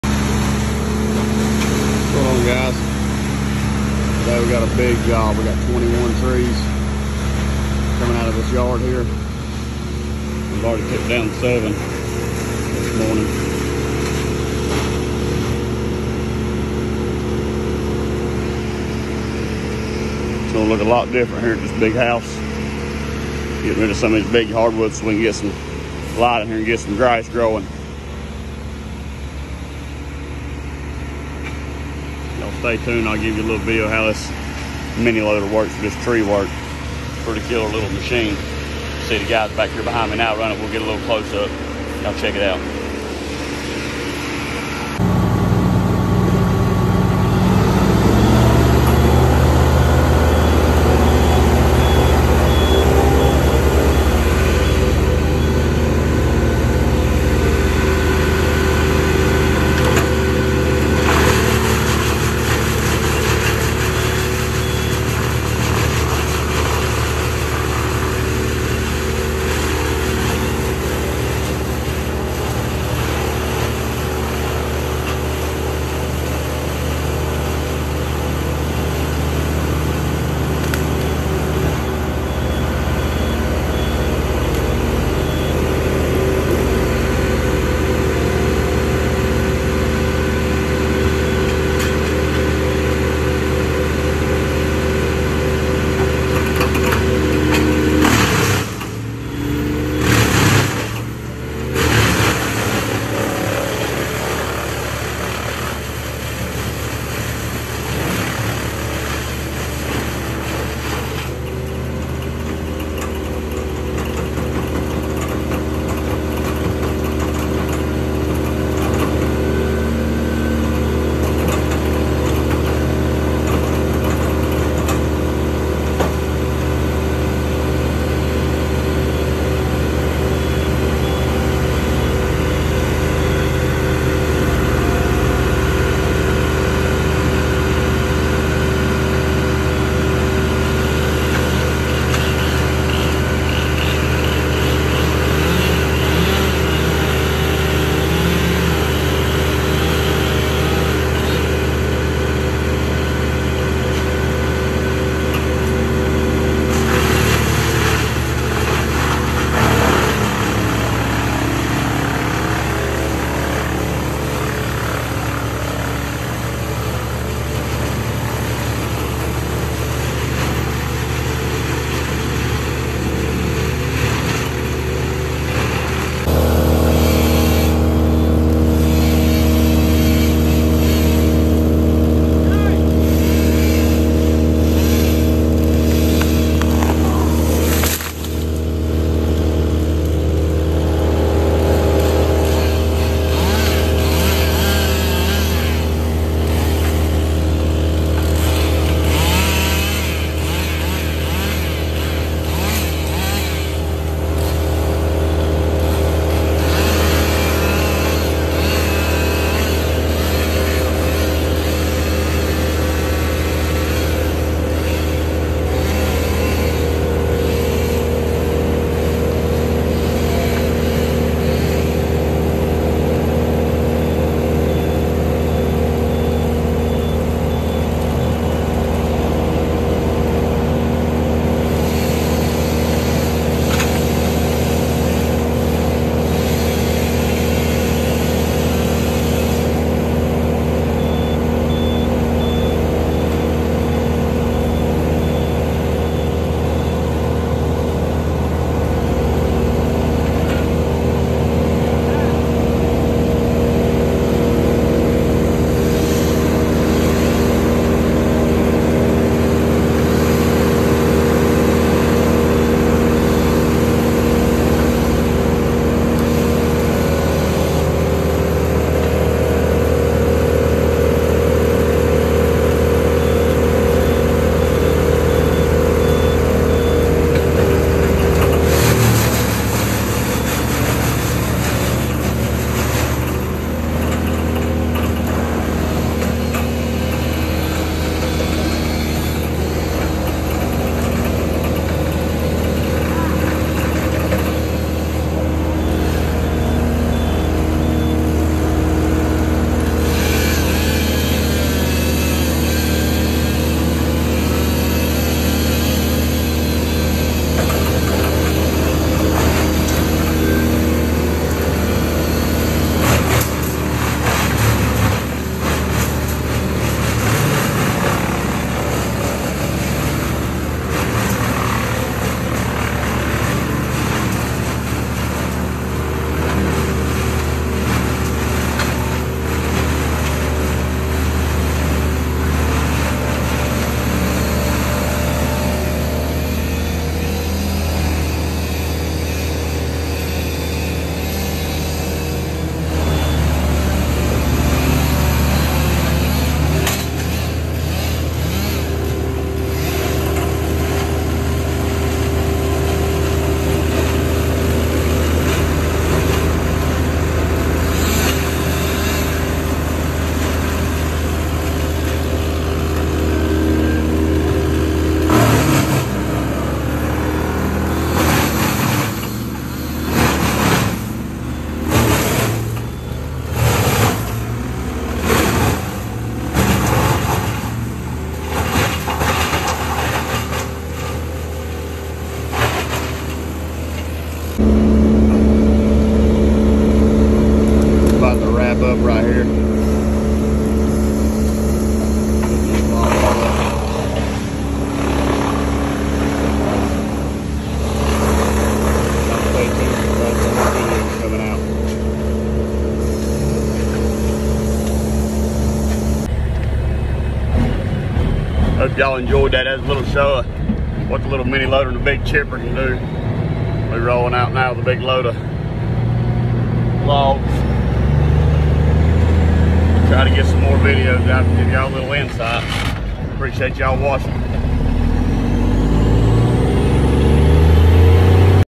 Tree work with a Mini loader and a 300hp Brush Bandit Chipper!
Short video of mini feeding the big brush bandit chipper on a job removing hardwoods